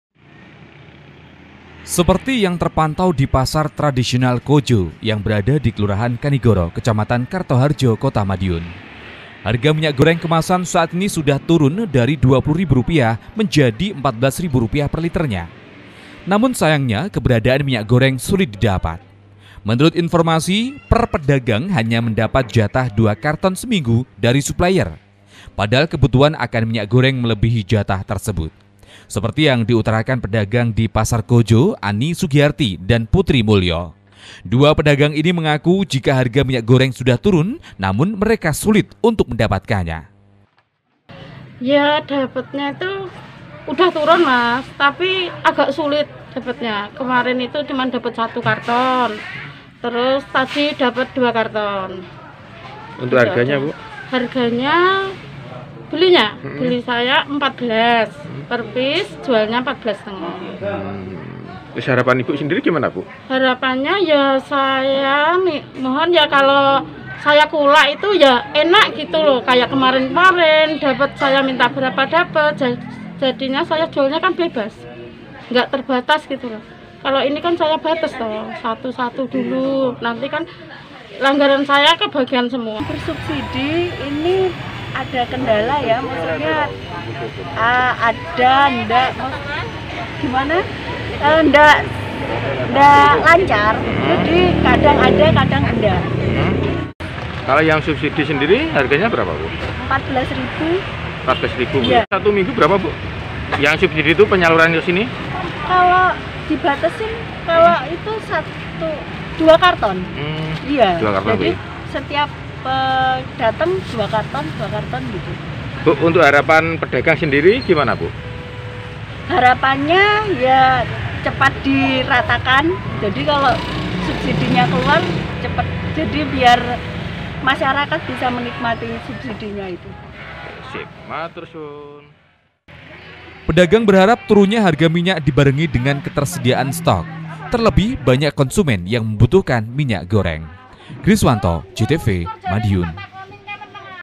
KOTA MADIUN - Harga Minyak Goreng Di Pasaran Sudah Turun, Tetapi Langka
Harga minyak goreng di sejumlah pasar tradisional di kota madiun sudah mulai turun. Dari 20 ribu rupiah menjadi 14 ribu rupiah perliternya. Namun keberadaannya sulit didapat.